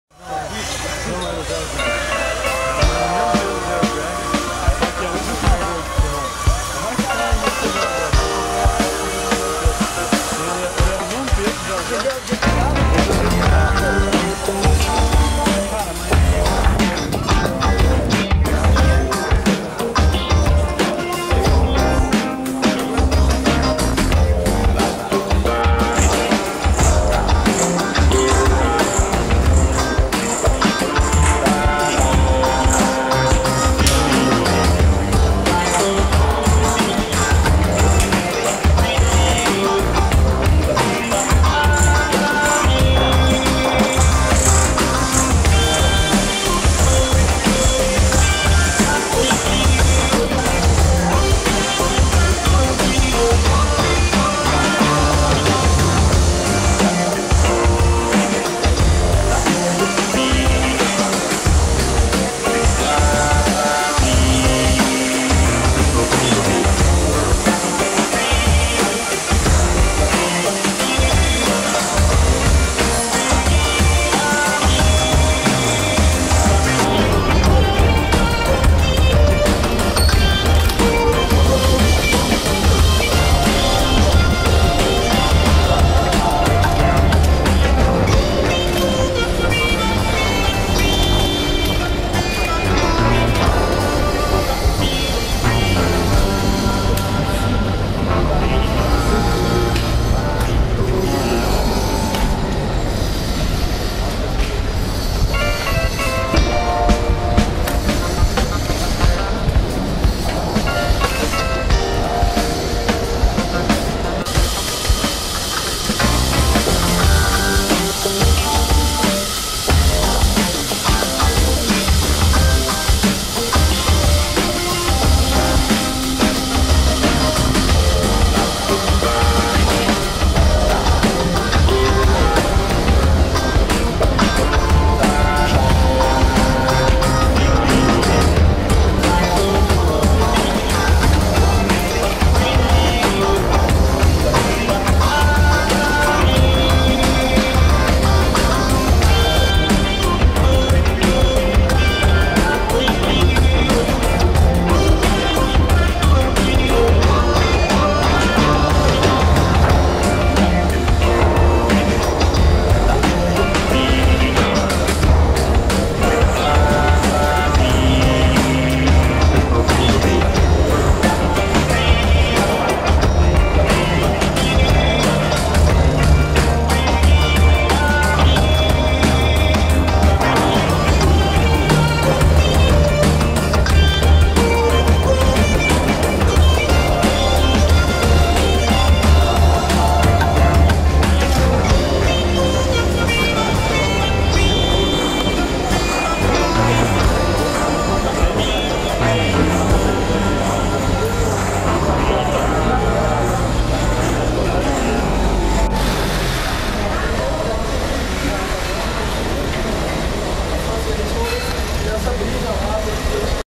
Maracanã Obras Novembro de 2012
Mais uma visita oficial com representantes da Fifa ao Maracanã. As obras começaram a entrar em fase de detalhes, cabos, gramados e finalização, que demorou bastante tempo. No início de 2013, as visitas não foram mais permitidas.